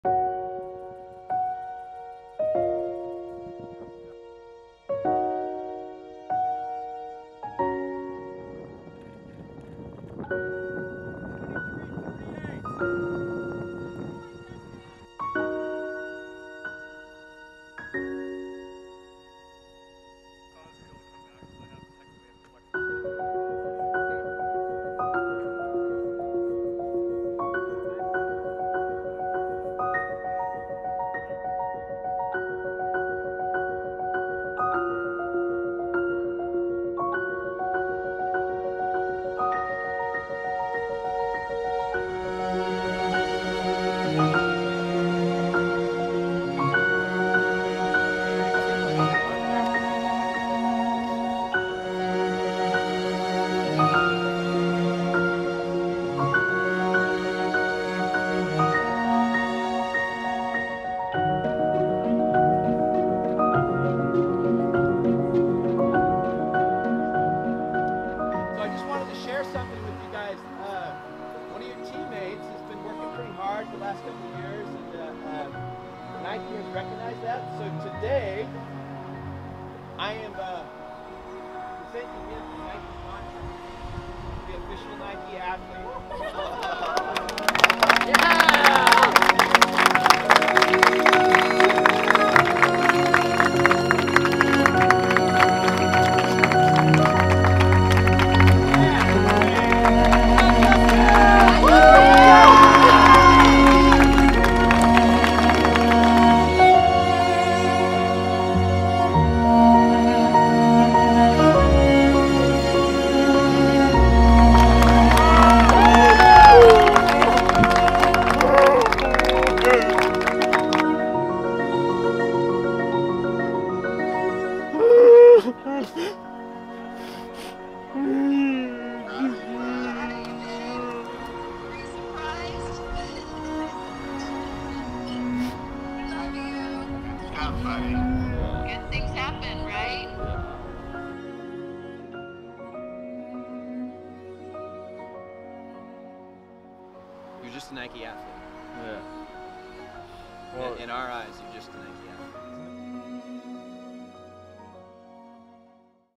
Justin Gallegos
Watch as Justin gets a huge surprise becoming a professional athlete.